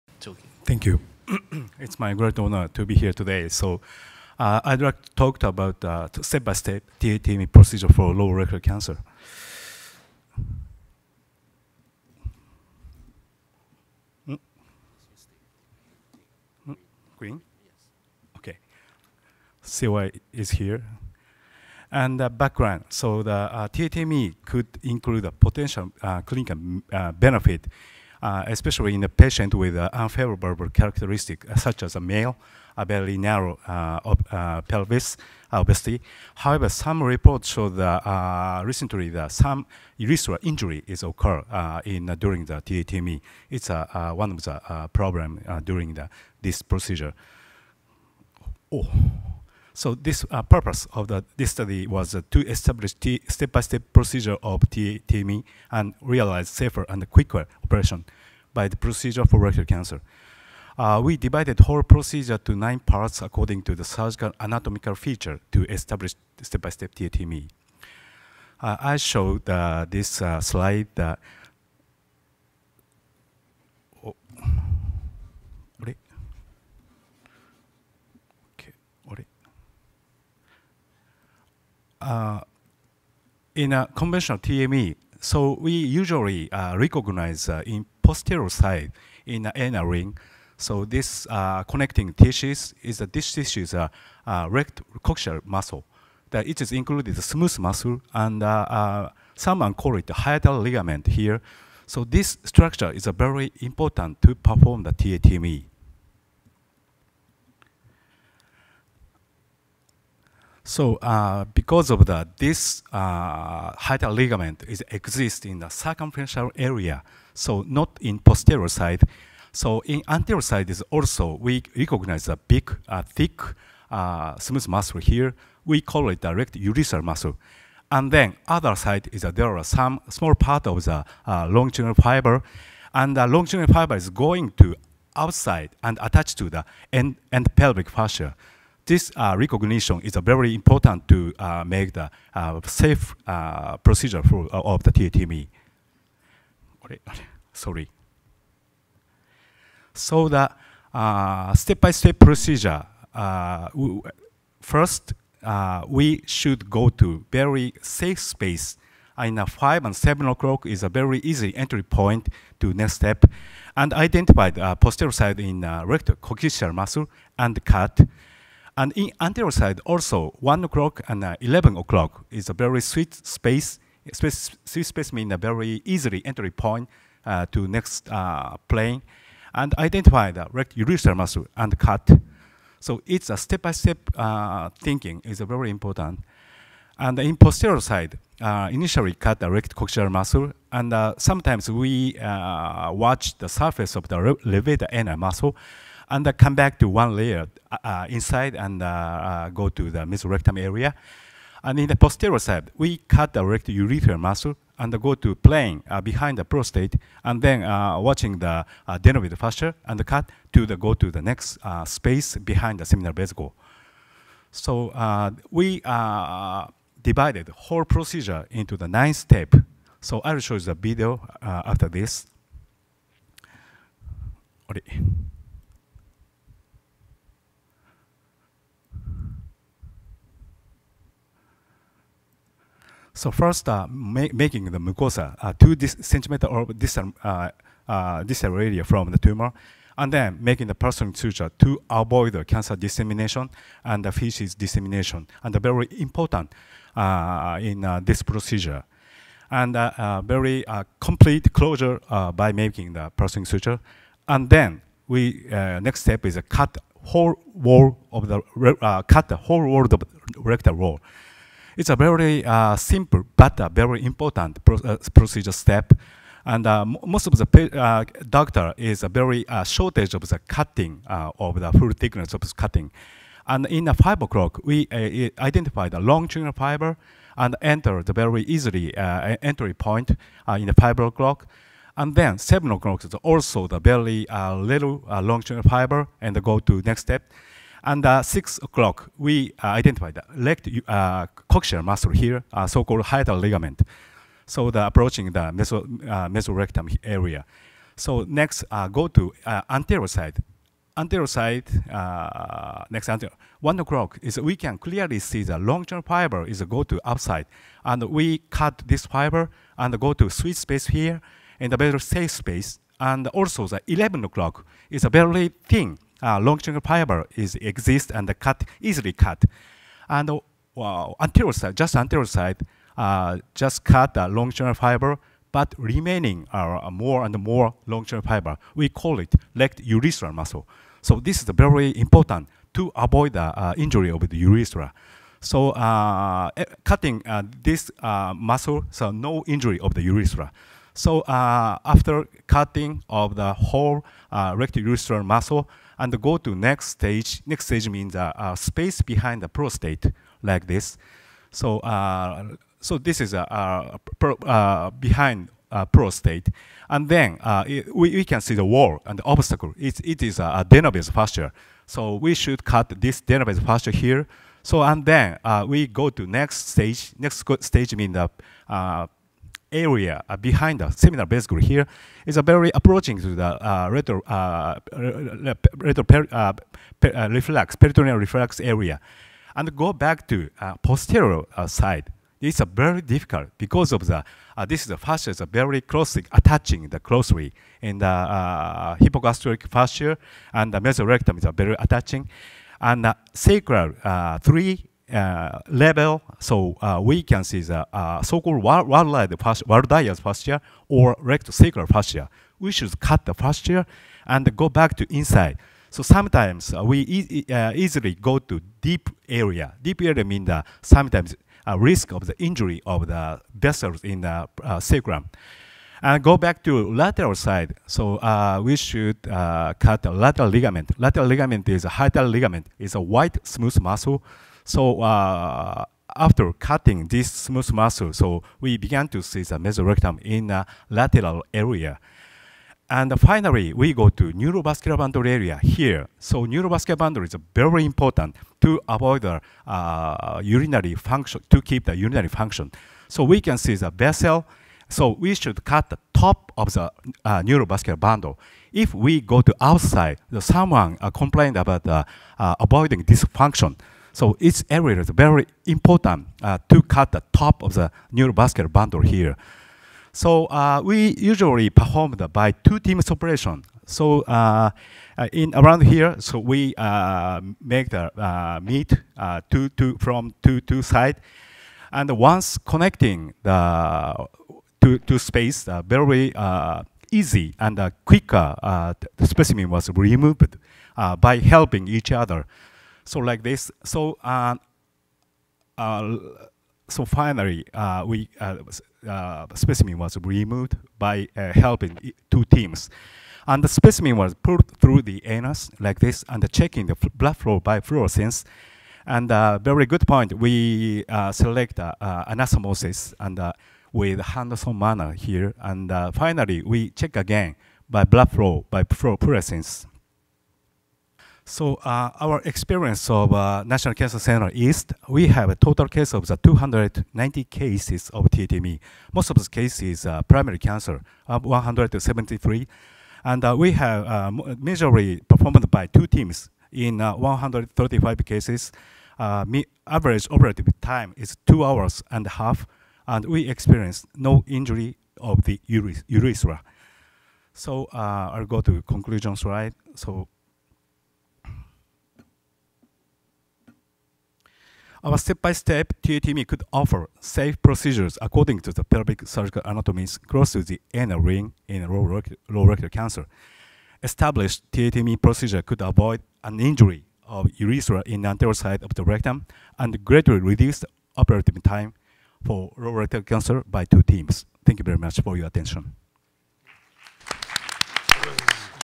Step-by-step procedures of taTME according to pelvic surgical anatomies for low rectal cancer
This talk was presented at the 2018 SAGES Meeting/16th World Congress of Endoscopic Surgery by Masaaki Ito during the SS28: Colorectal 2 Session on April 13 2018